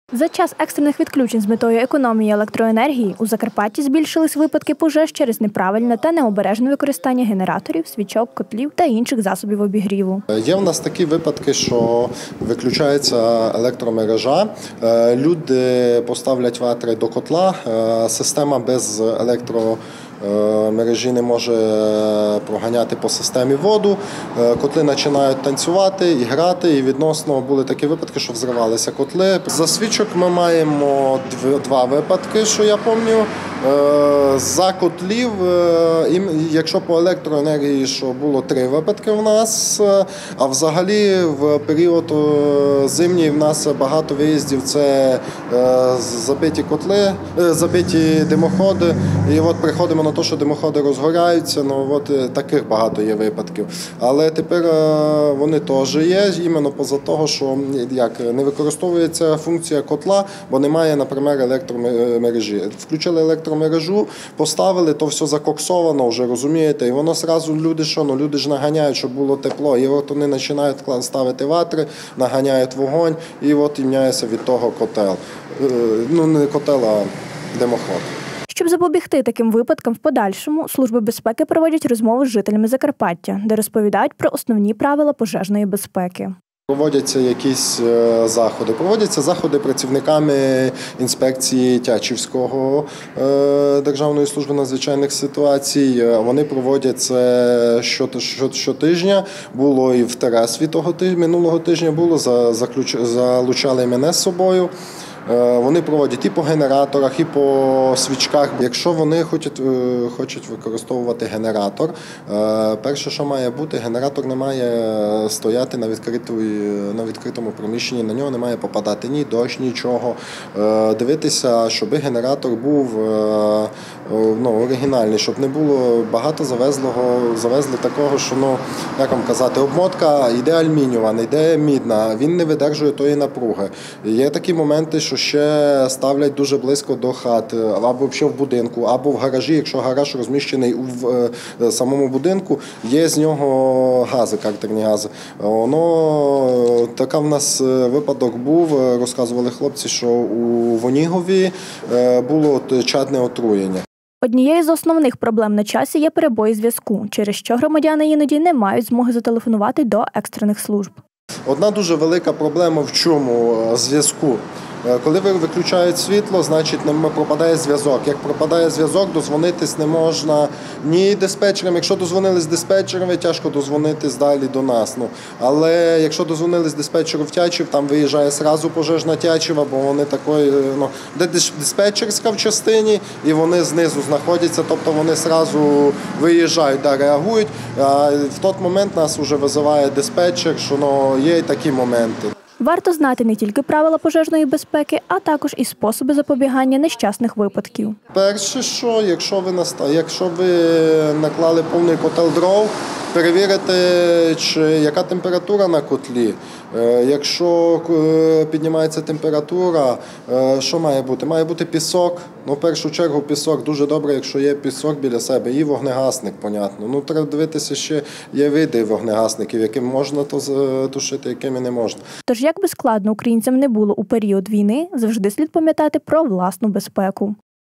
Небезпека від засобів обігріву
За час екстрених відключень з метою економії електроенергії у Закарпатті збільшились випадки пожеж через неправильне та необережне використання генераторів, свічок, котлів та інших засобів обігріву. Щоб запобігти таким випадкам в подальшому, служби безпеки проводять розмови з жителями Закарпаття, де розповідають про основні правила пожежної безпеки.   Однією з основних проблем на часі є перебої зв’язку, через що громадяни іноді не мають змоги зателефонувати до екстрених служб.Варто знати не тільки правила пожежної безпеки, а також і способи запобігання нещасних випадків. Тож якби складно українцям не було у період війни, завжди слід пам’ятати про власну безпеку.